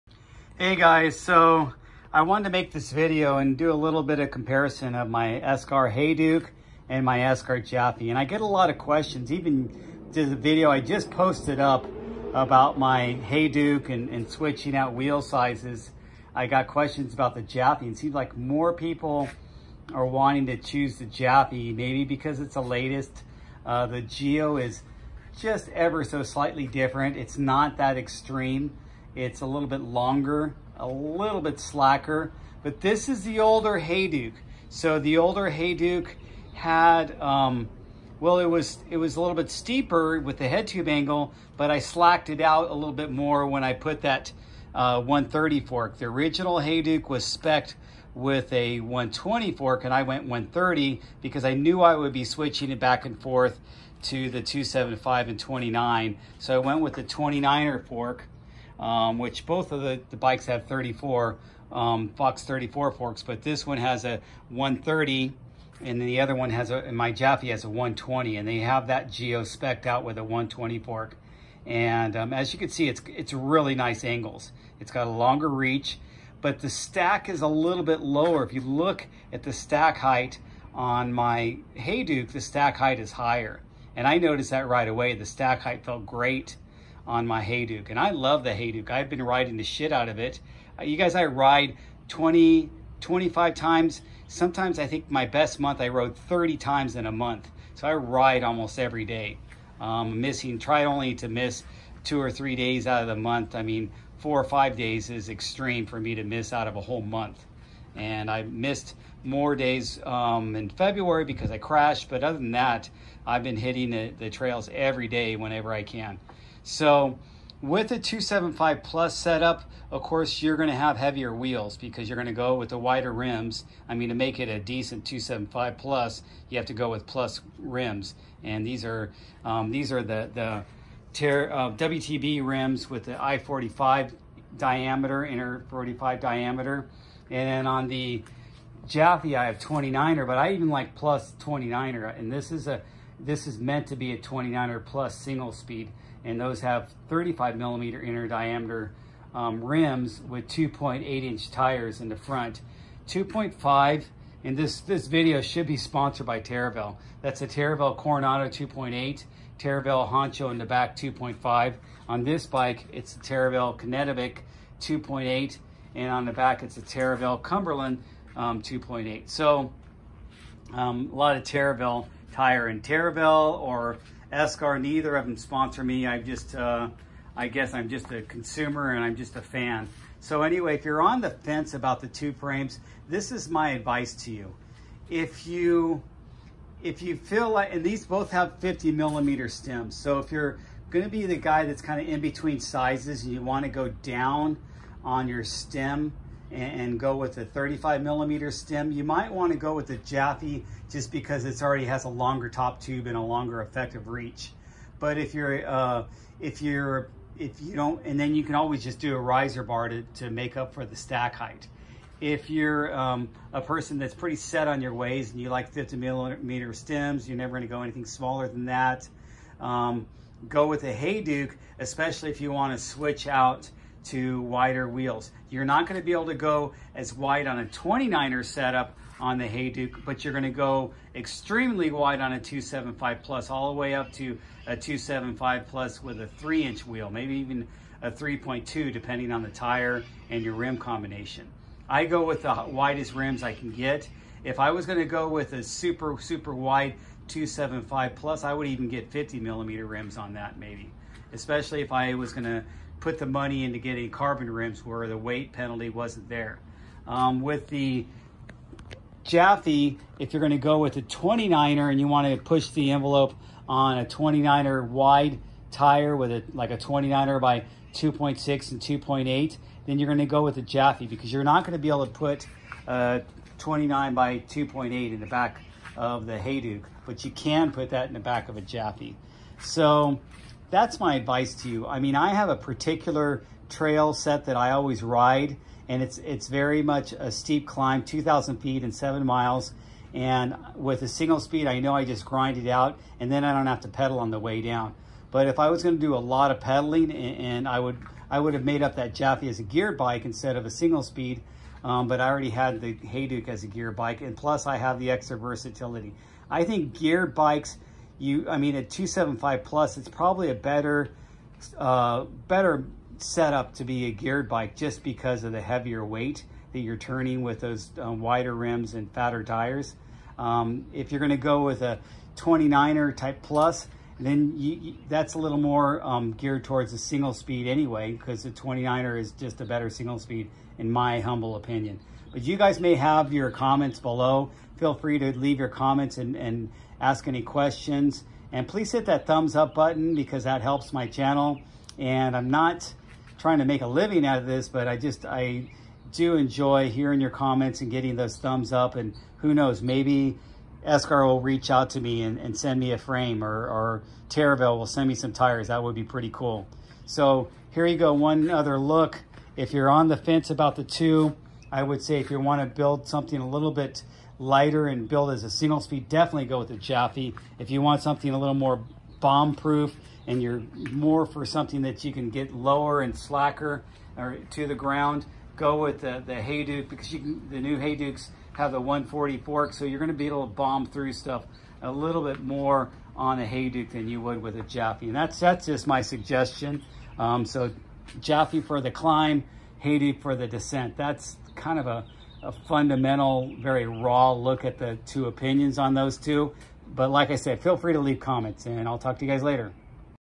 Mini comparison of Esker Hayduke and Japhy with 34Fox forks
Just a brief discussion on the differences between the Hayduke and Japhy and some of my opinion why you would want either one for your trails and riding style